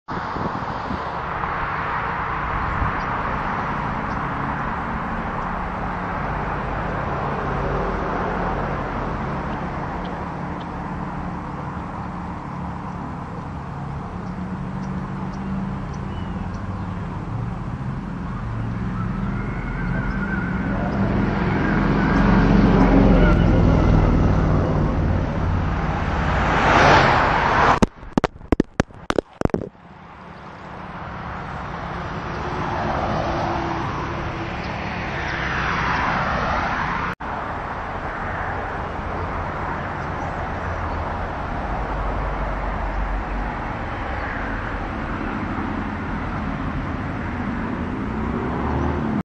Just my goro getting ran over, then me running in heavy ass gear towards it. Felt like an astronaut
Just my goro getting ran over, then me running in heavy ass gear towards it. Felt like an astronaut. Magnet mount didn't grab well enough to the bike